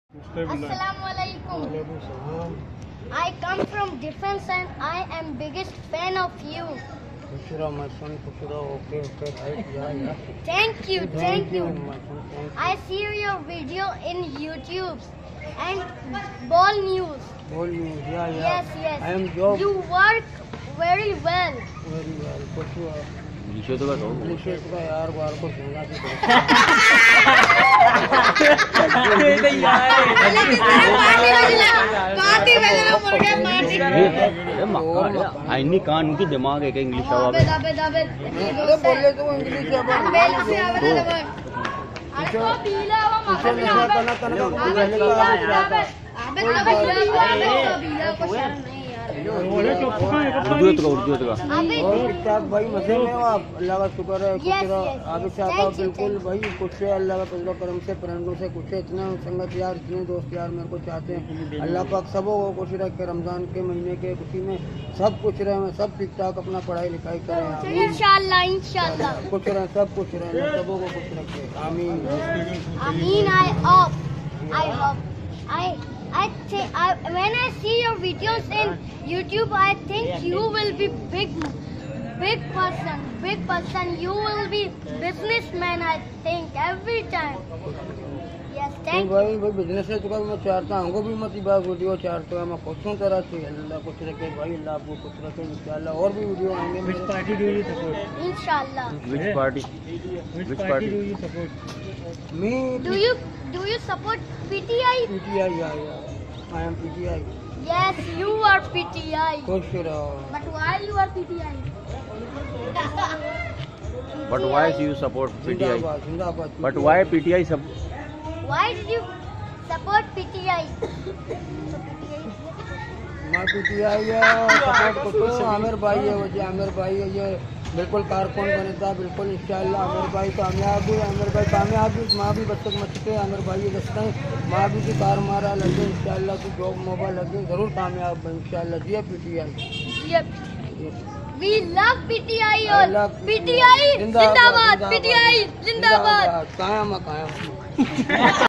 Abid Shaka Ka Chota Fan Dekhe Kesi English Bolta Hai (Geo mera Chota Fan)
Abid Shaka Se bache Bhot Piyar Karte Hai Abid Shaka Sab Ka Hero Hai Kiya Chota Kiya Bara Kiya Larki Kiya Larka Sab Abba Log Hai Apne Sab o Ko Allah Pak Kush Rakhe Kam Me Barkat De Ameen...!!!